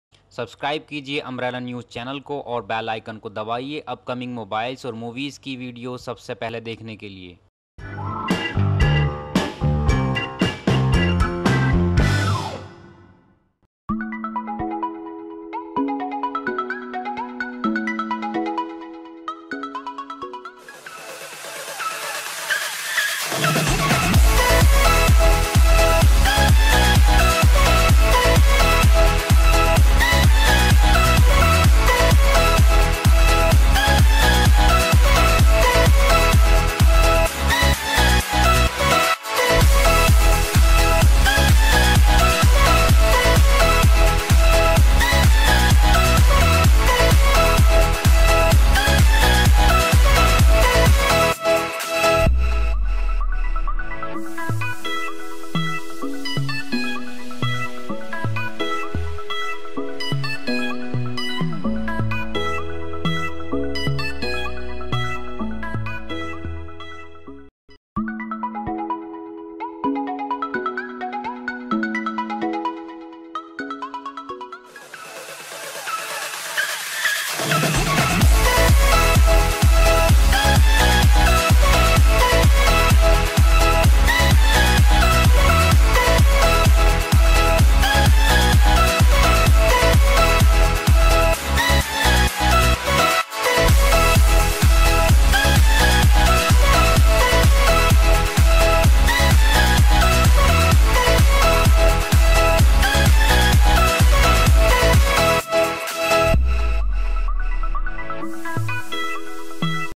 Nokia 9 Review & Specs
PERFORMANCE
Qualcomm Snapdragon 835
4 GB RAM

DISPLAY
5.2 & 5.5 inches HD Display

CAMERA
12 MP camera with flash
12 MP Front Camera

BATTERY
Unkown mAh

OTHERS
64 GB Internal
4G Supports
Dual SIM